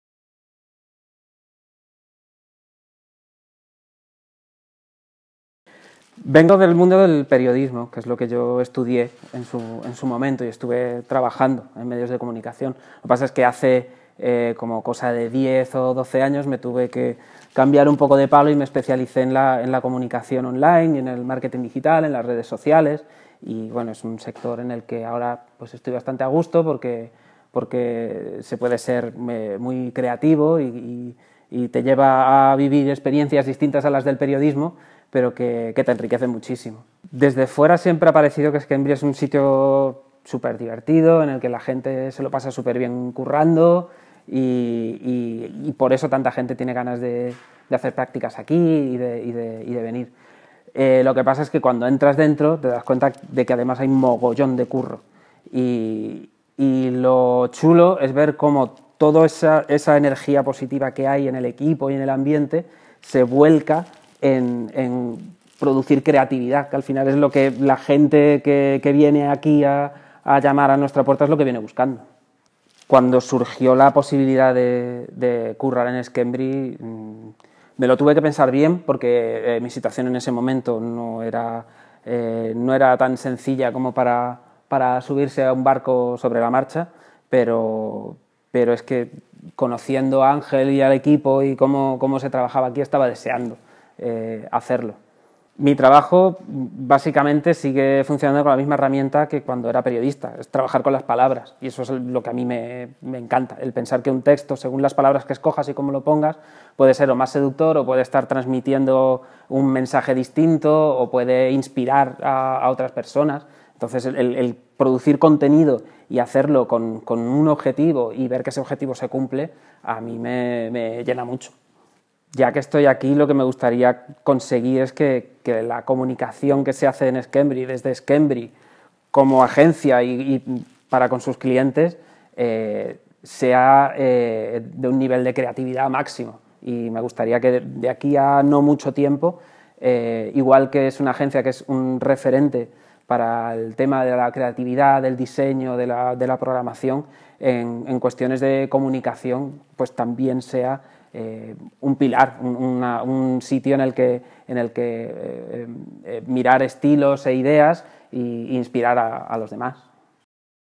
Luis Arronte es un Squembri
Periodista, profesor, redactor y copy. Lo suyo son las palabras, desde hace más de veinte años (sí, parece más joven). Es el Director de Comunicación de Squembri.